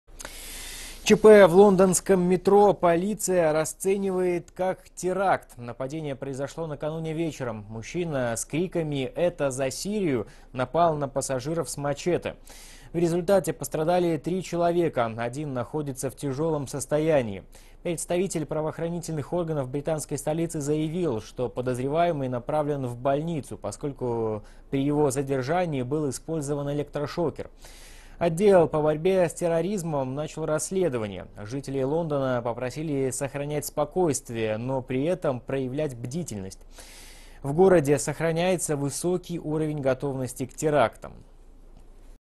Три человека пострадали при нападении мужчины с мачете в метро Лондона
Атака в лондонском метро